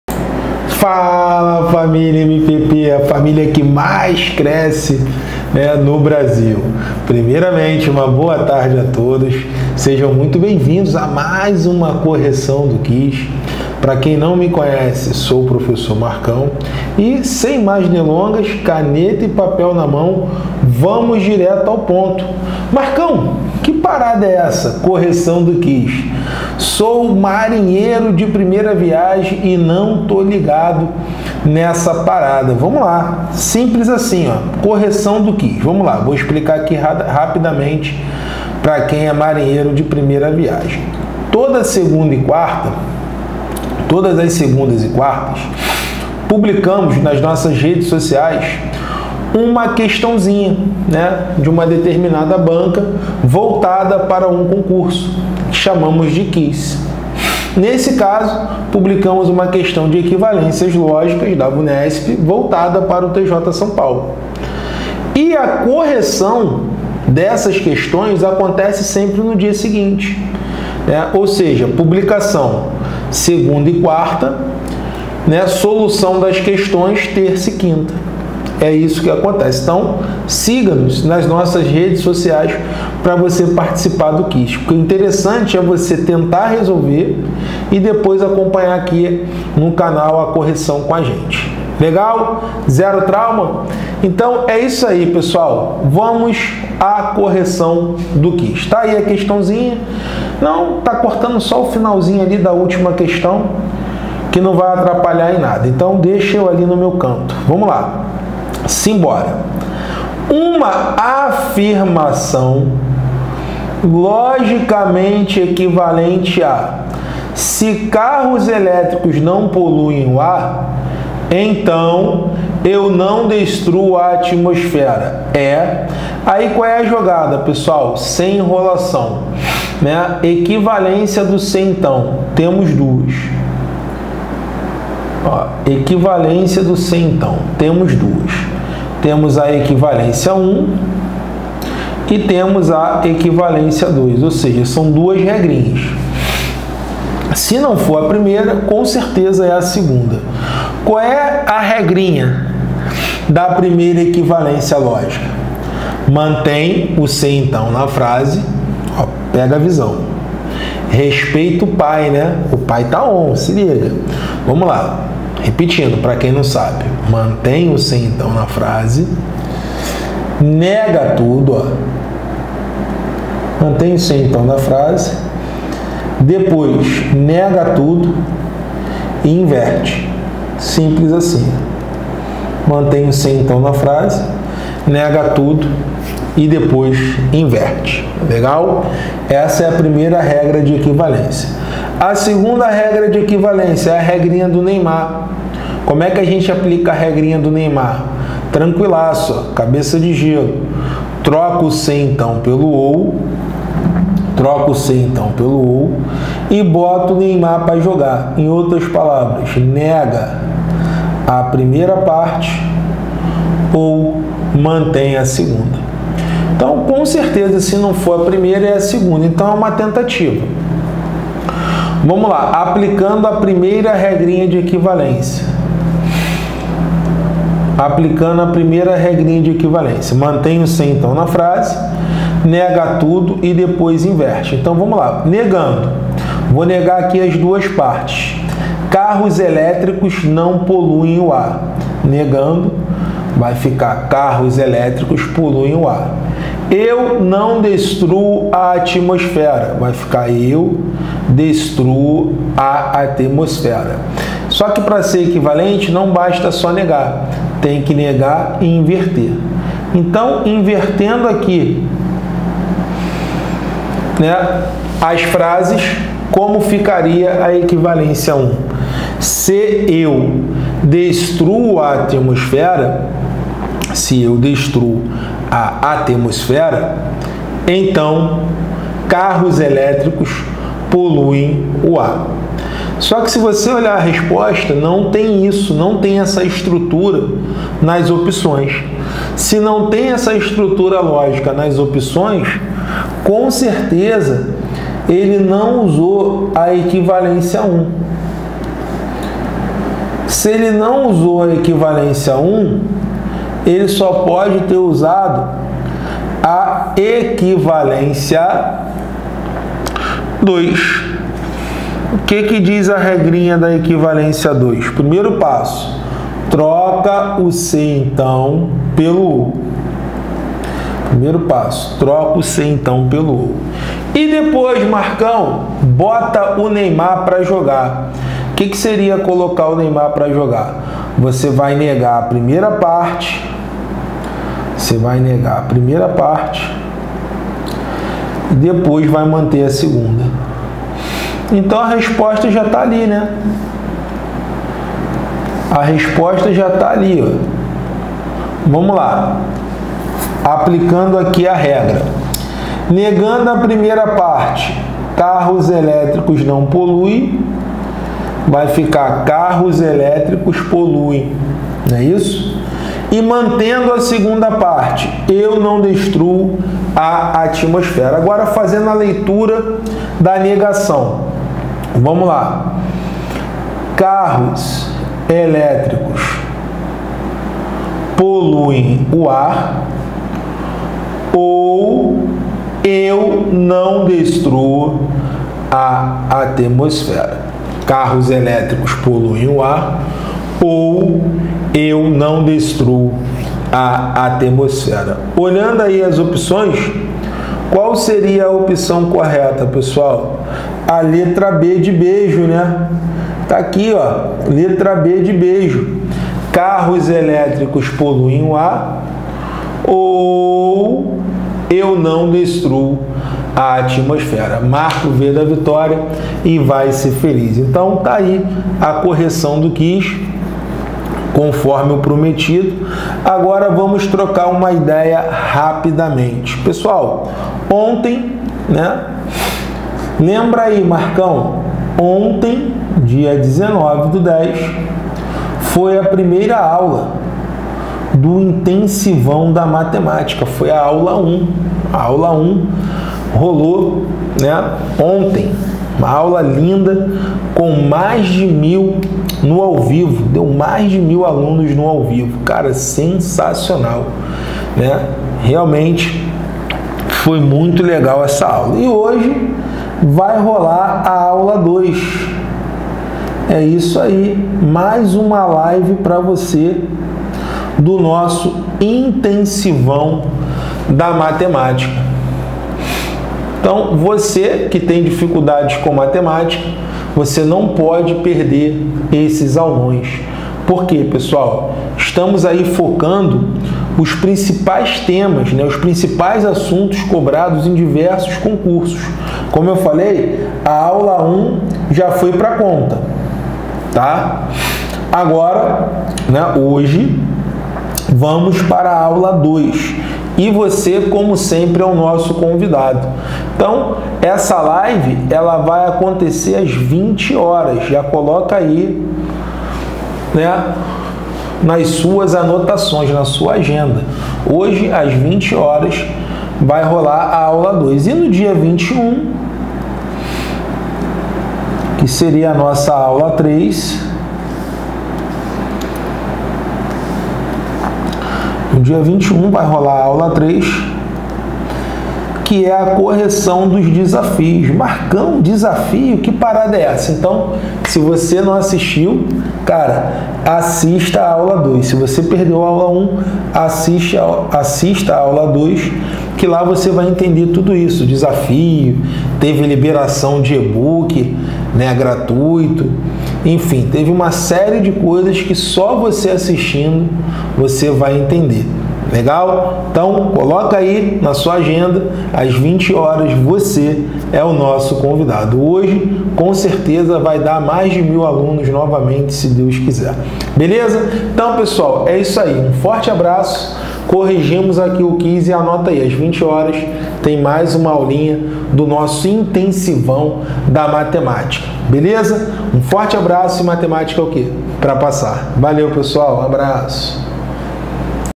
Correção do Quiz : Tudo que você precisa fazer sobre equivalências lógicas
Você está estudando Equivalências Lógicas?

Então assista esse vídeo onde nosso professor,Marcão, ensina como Aprender as Equivalências Lógicas e não decorar, você verá que depois desse vídeo as questões de Equivalências serão bem mais tranquilas.